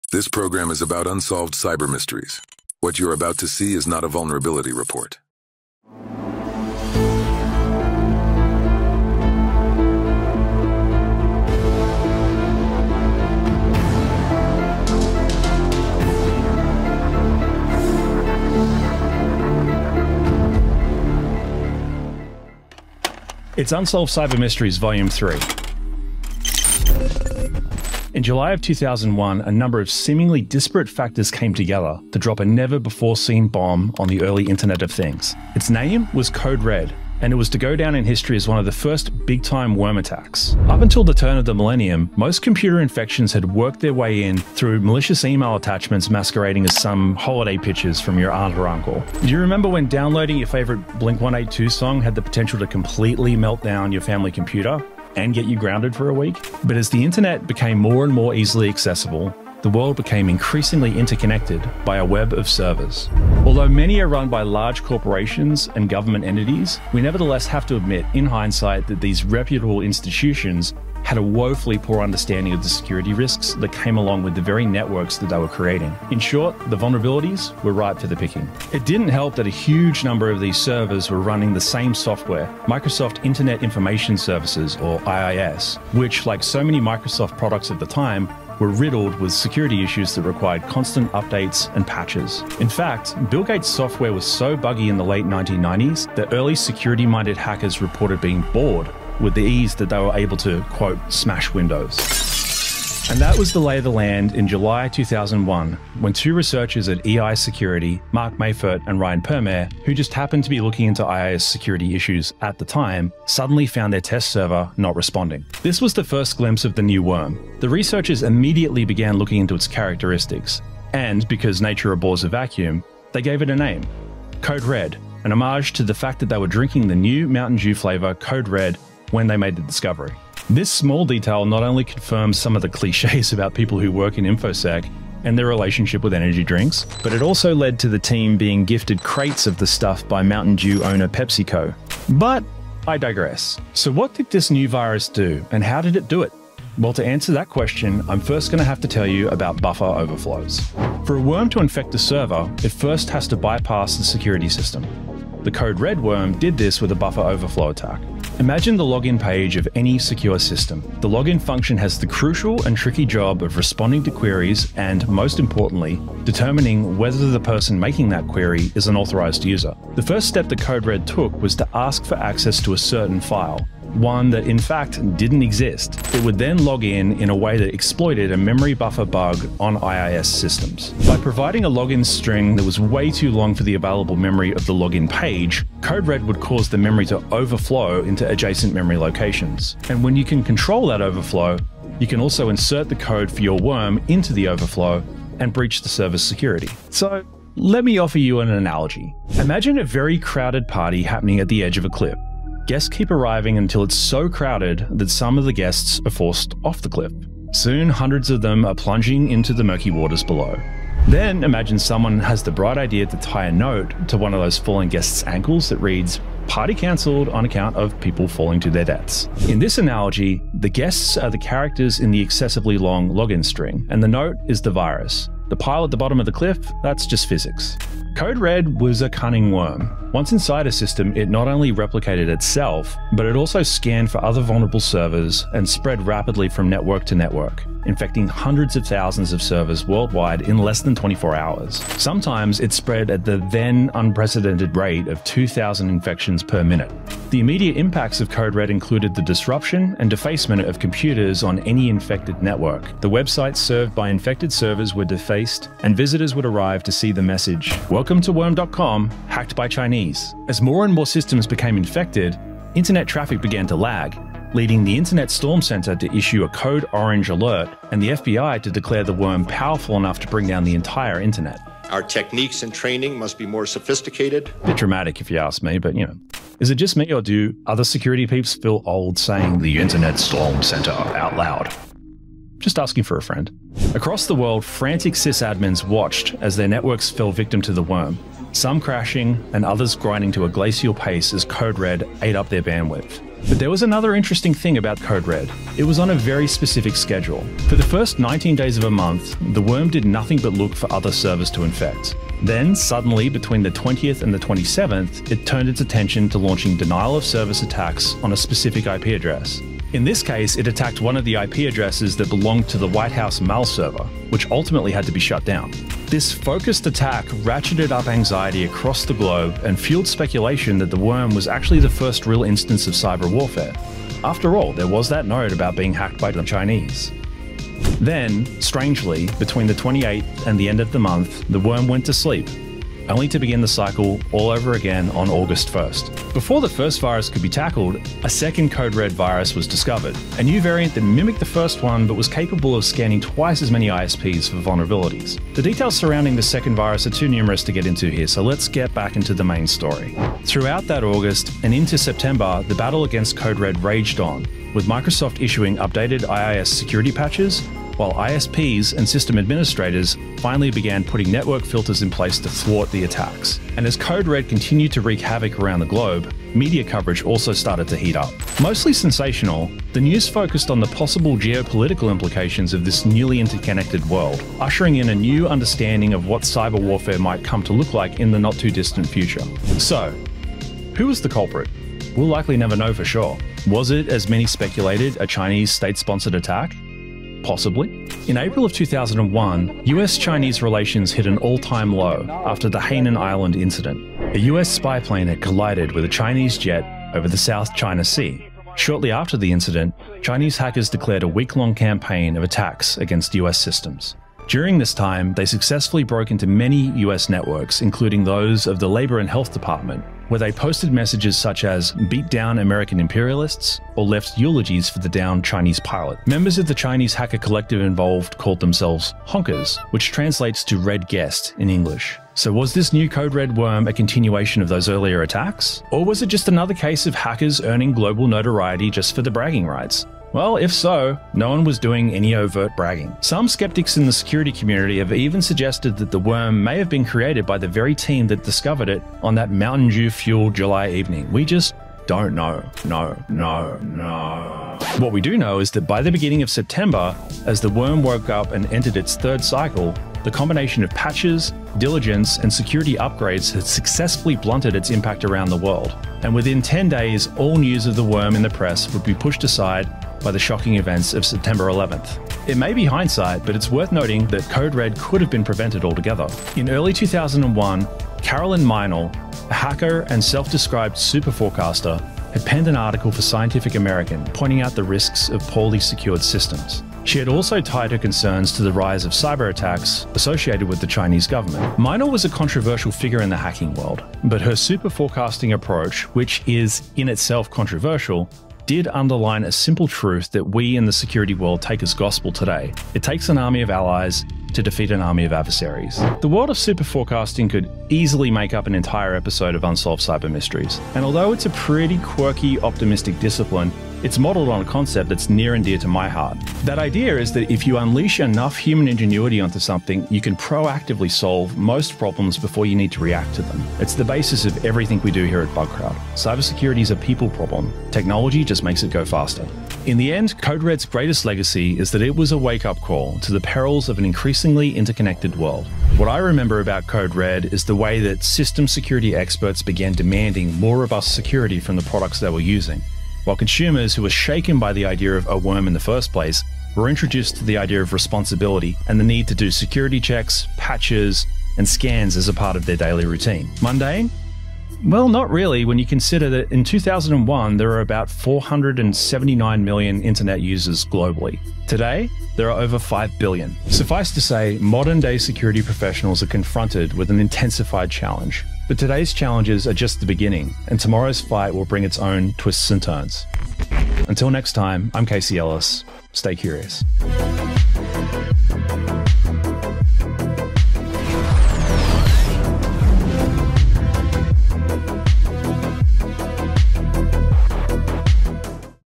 Unsolved Cyber Mysteries with Casey Ellis • Volume 3
In this episode, a relentless cyber worm unleashes global havoc, exposing the vulnerabilities of an internet-connected world. Experience the pulse-pounding race against time as hackers, government officials, and ordinary internet users grapple with the emergence of cyber warfare.